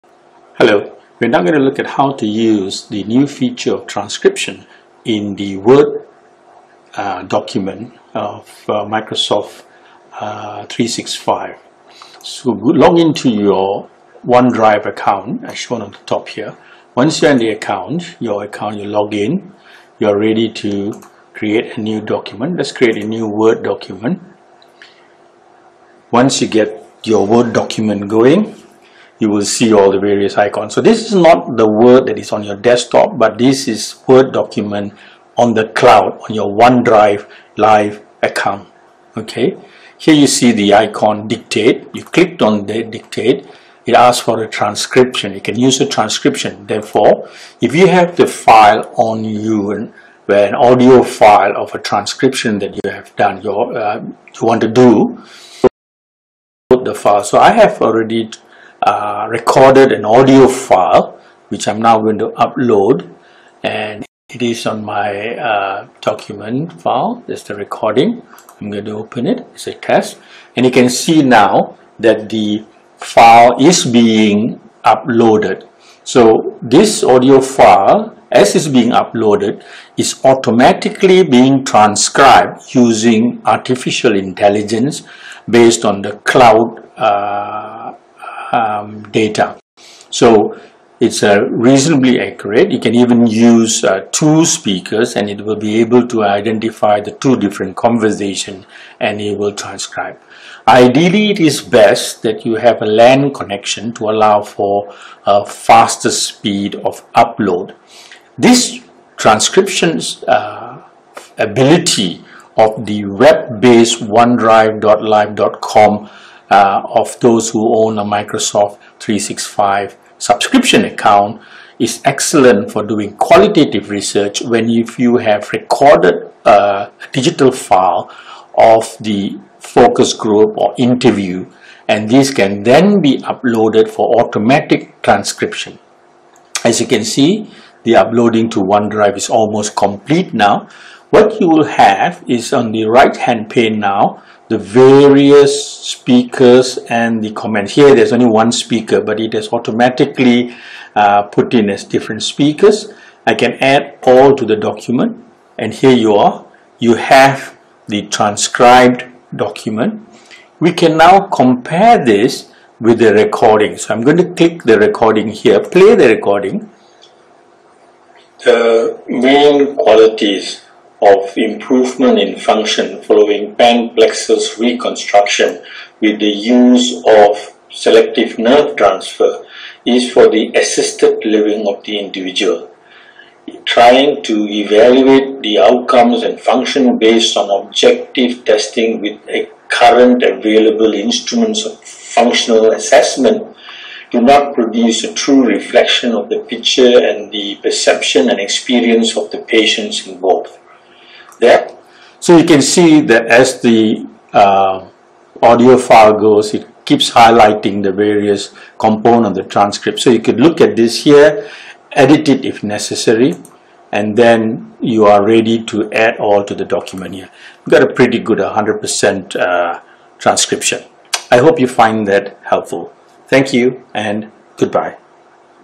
Audio transcription feature in Word
The online version of Word for Microsoft 365 subscribers, with an easy way to automatically transcribe audio.And even the ability to record conversations directly